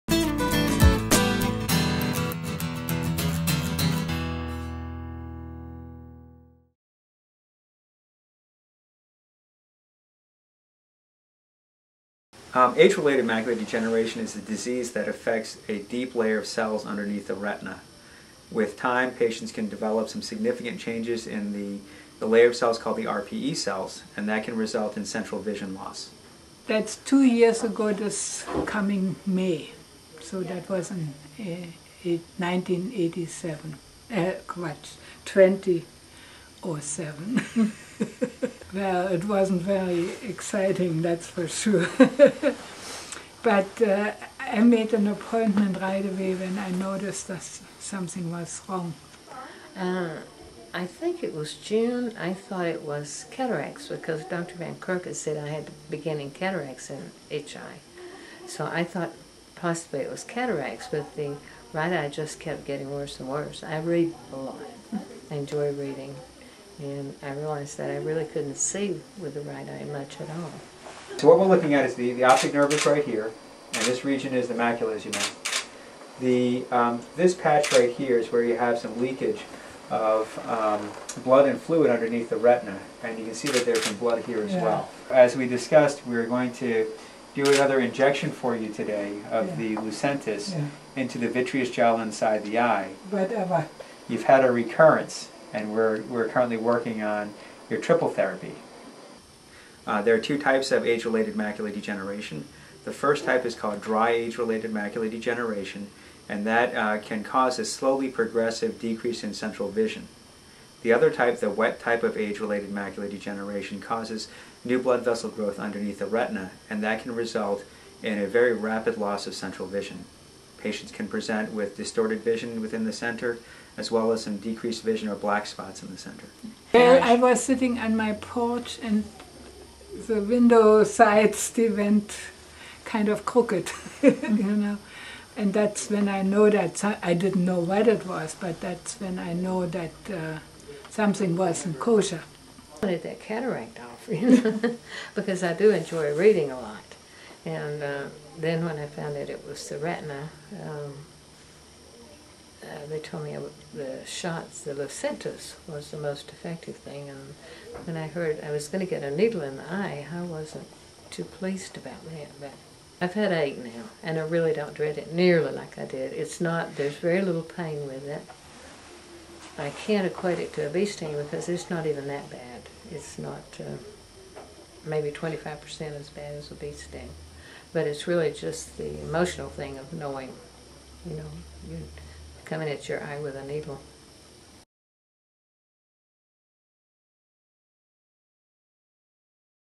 Macular Degeneration
Dr. Robert Park, retina specialist with Carolina Ophthalmology, and two of his patients discuss age-related macular degeneration.